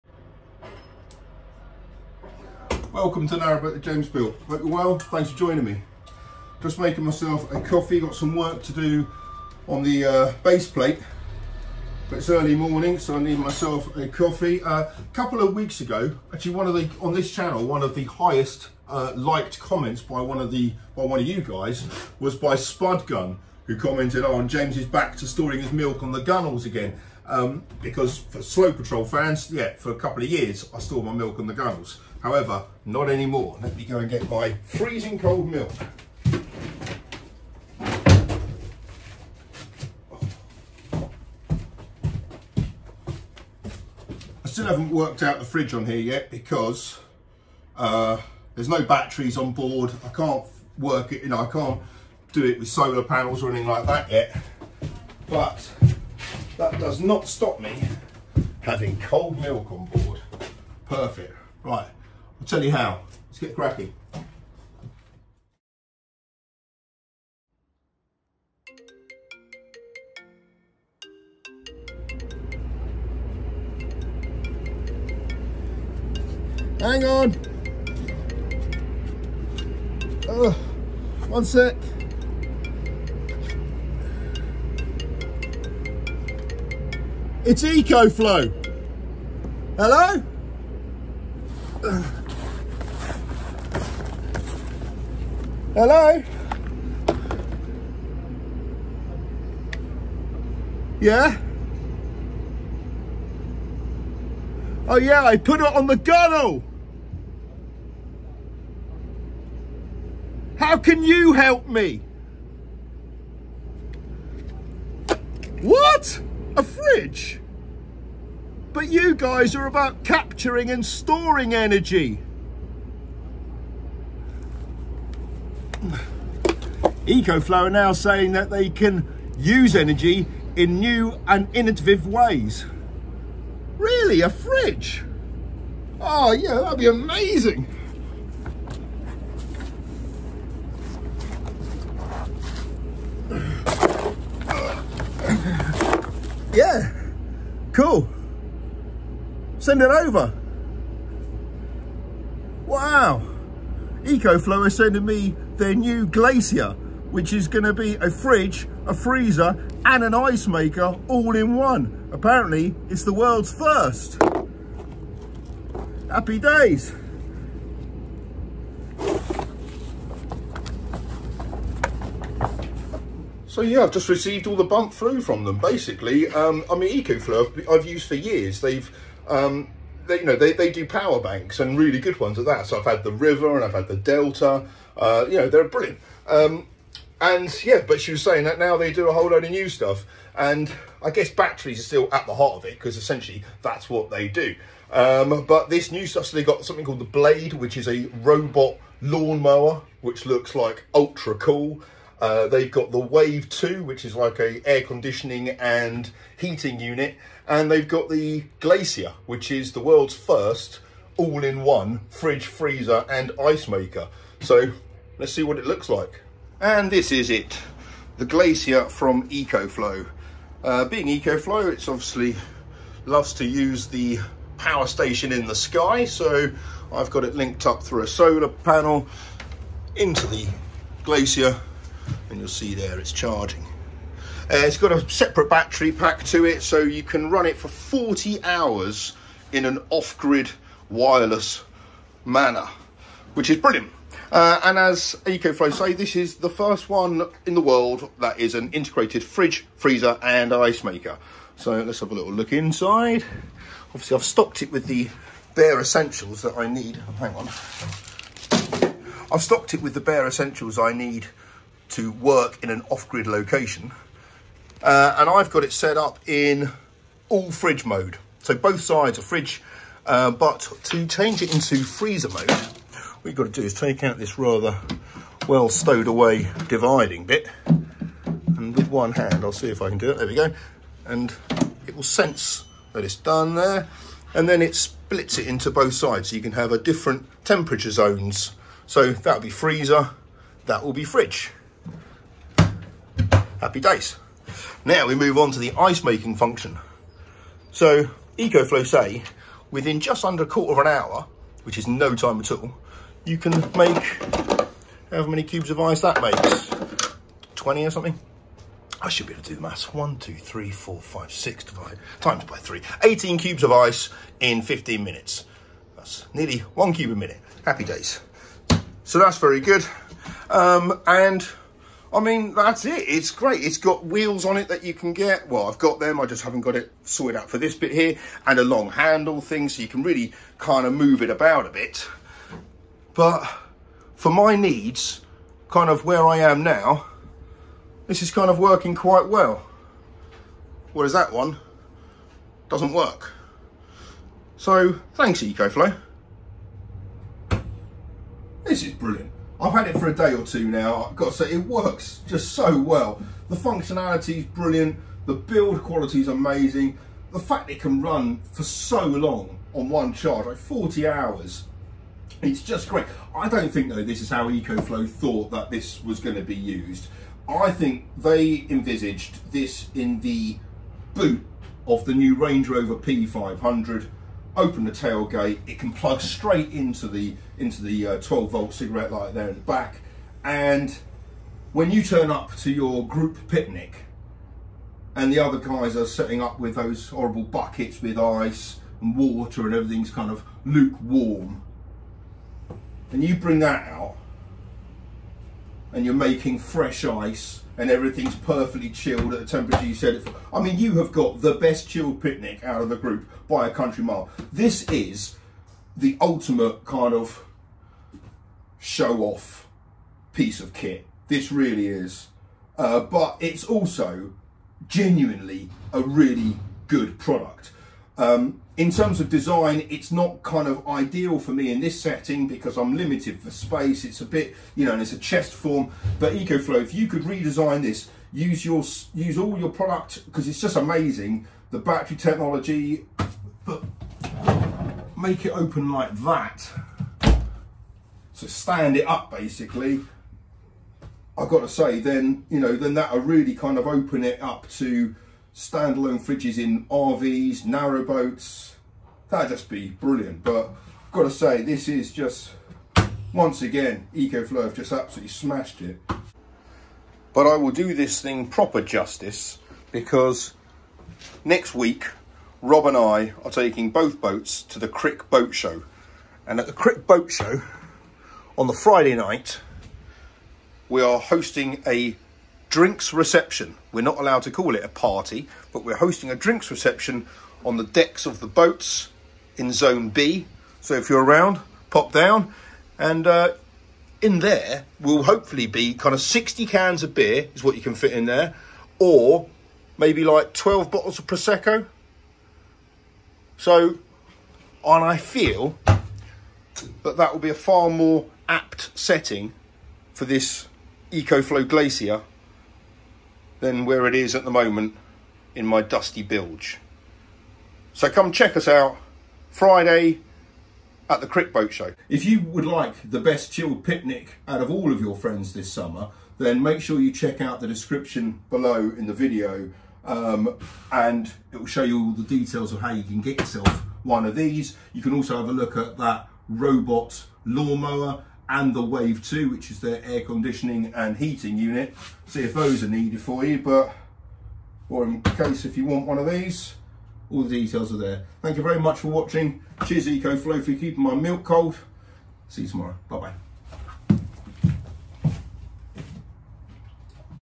436. No more milk on the Gunnels say ECOFLOW
EcoFlow said that putting my milk on the gunnels is just not appropriate for a modern day influencer and have sent me the Glacier which is a worlds first portable wireless fridge freezer and ice maker all in one.

World's first 3-in-1 portable fridge-freezer with an integrated ice maker.